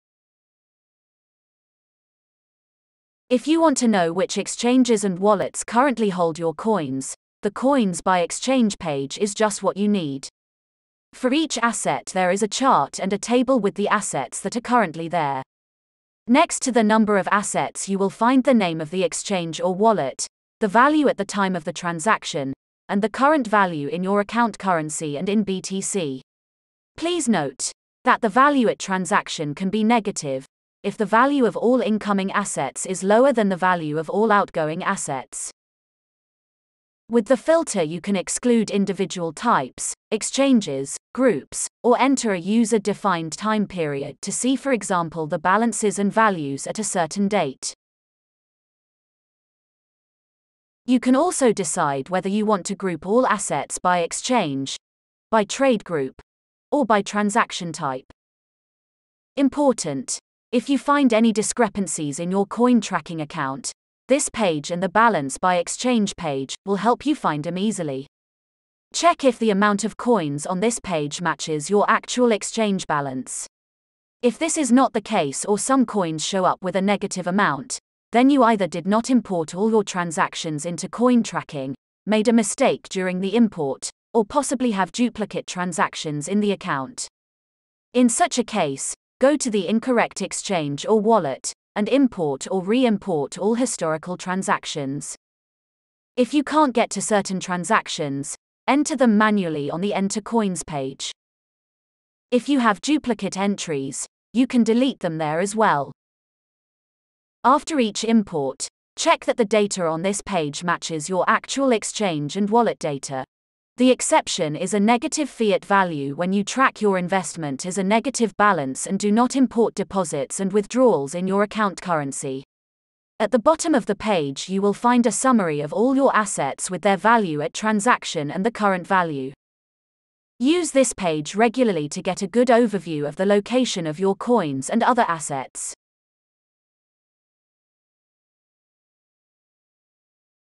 CoinTracking Reports: Coins by Exchange
The location of all your coins, assets and currencies with their amount and value by exchange, transaction type or trade-group.

Learn how to use the Coins by Exchange report from CoinTracking. Find all the details for your coins, assets, and currencies by exchanges or wallets ⬇️

Amount of each currency by exchange or wallet with charts
Value at transaction time for each exchange/wallet
Current value for each exchange/wallet
Grouping by exchange, trade-group, or trade type
Many other filtering options (e.g., time periods, exchange filters, transactions)
Summary of all your assets with the number of exchanges, value at the transaction time, and current value

🎥 More videos showing you how to take advantage of all the CoinTracking features